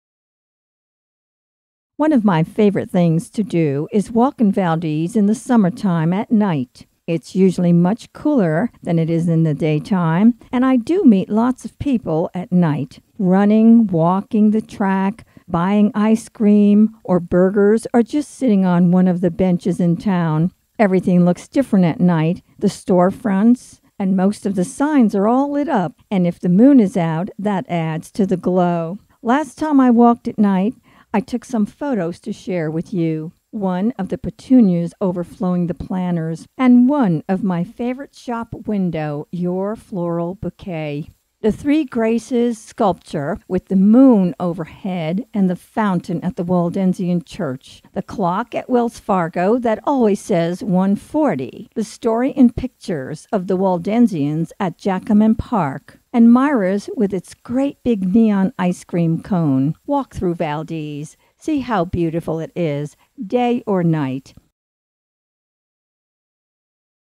Walking in Valdese at Night
Presented by Rose Mueller
Enjoy a summer evening in Valdese by walking our downtown.